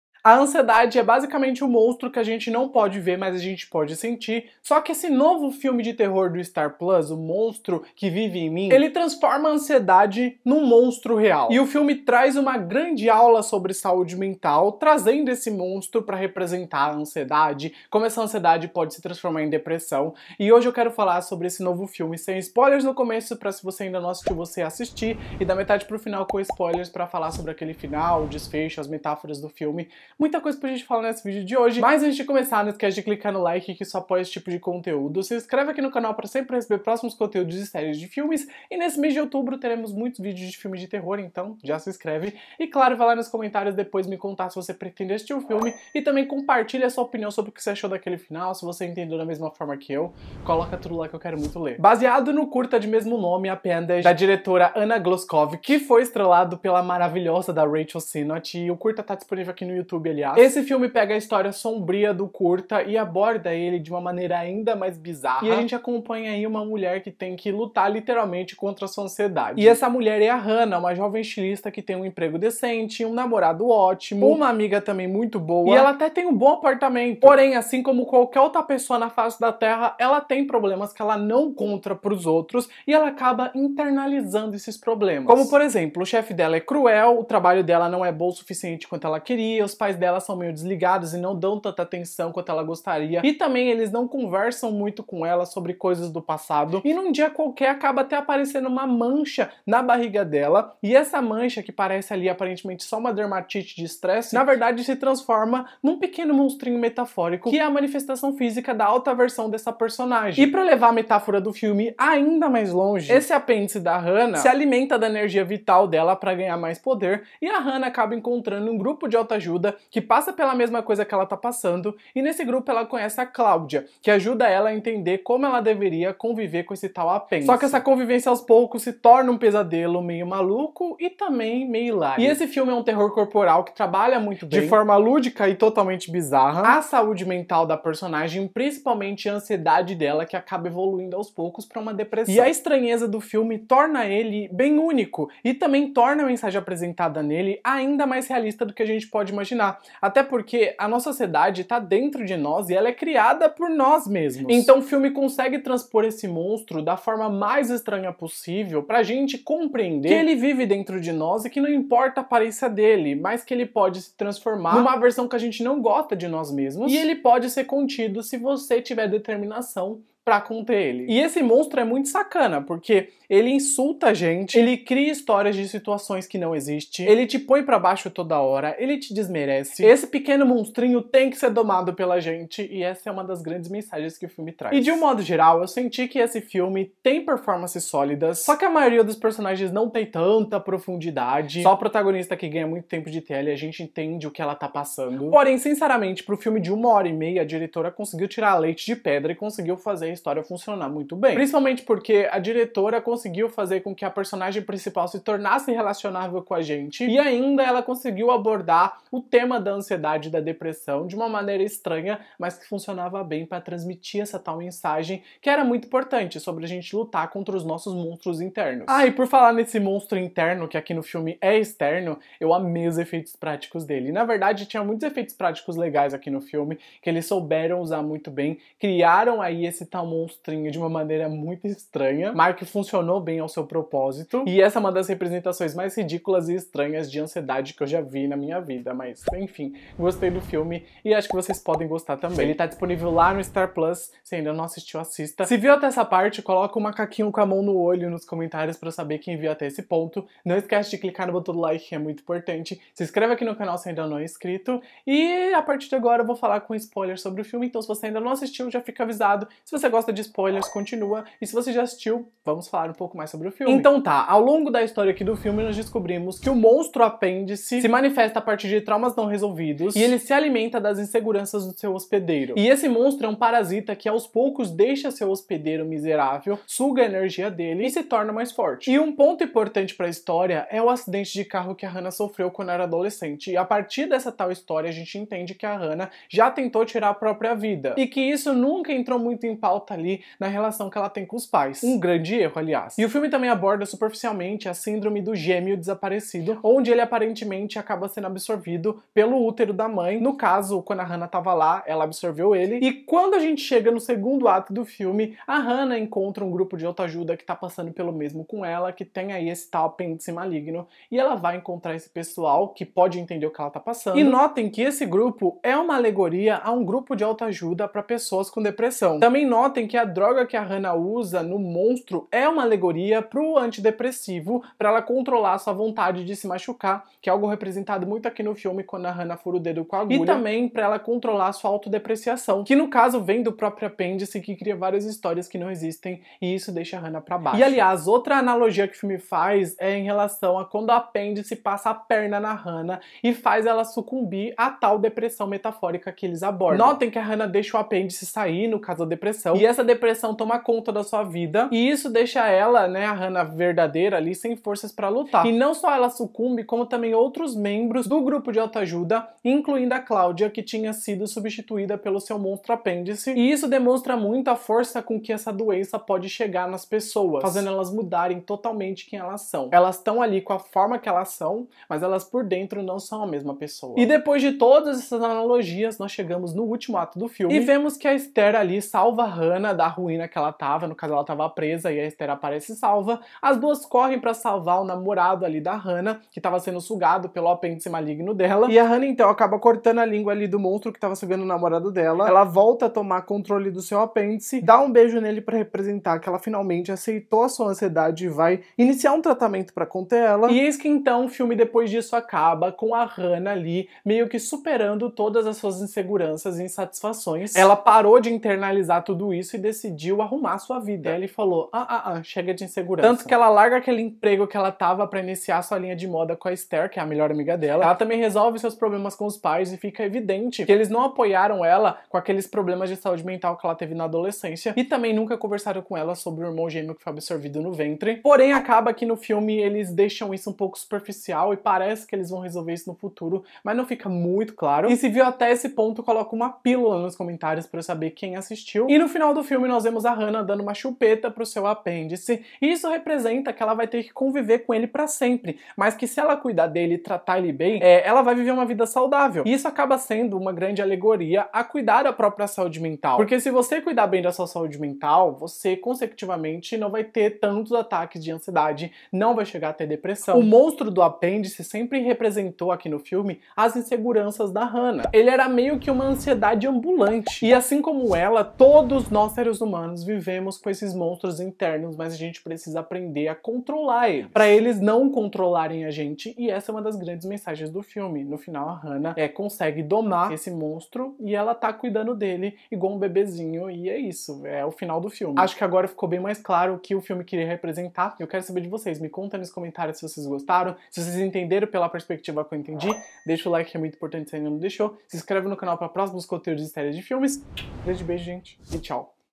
O Monstro que vive em mim | Critica + FINAL EXPLICADO - Terror sobre Apêndice Maligno
O Monstro que vive em mim (Appendage) chegou no Star+ e aqui está uma Critica + FINAL EXPLICADO sobre esse terror corporal, sobre um apêndice Maligno.

Sinopse: Hannah ( Hadley Robinson ) é uma estilista que está se esgotando rapidamente, em parte graças ao ambiente tóxico de seu chefe esnobe, Cristean ( Desmin Borges ), e seus padrões inextinguíveis. Ela trabalha com sua amiga Esther ( Kausar Mohammed), e ambos têm tatuagens de pulso, uma imagem sucinta e significativa de como são dedicados ao bem-estar um do outro. Mas esse vínculo é testado pela dúvida em sua cabeça: Hannah não apenas investe na tentativa de satisfazer um chefe tóxico, mas também começa a pensar que seu namorado de seis meses, Kaelin (Brandon Mychal Smith), a está traindo com Esther. . Adicione sua tensão com sua mãe Stacy (Deborah Rennard), com quem ela esconde seus verdadeiros problemas mentais, e explode o apêndice de Hannah, tentando orientar Hannah mentalmente e depois fisicamente.

Sumário:
00:00 - Critica (Sem Spoilers)
04:16 - FINAL EXPLICADO